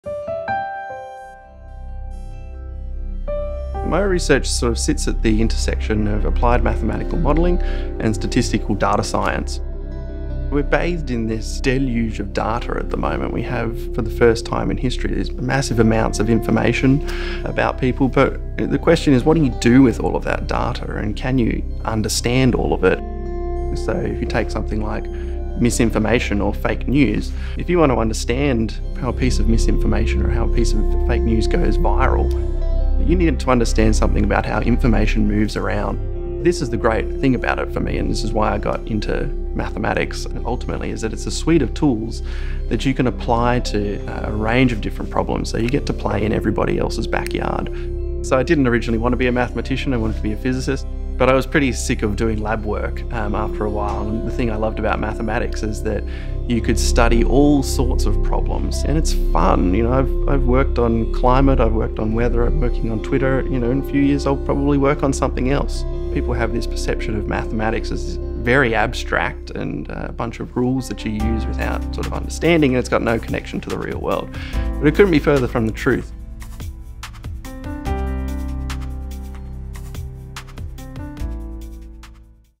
Dr Lewis Mitchell - Finalist Tall Poppy of the Year
2018 SA Science Excellence Awards.
Dr Lewis Mitchell, Lecturer in Applied Mathematics, The University of Adelaide.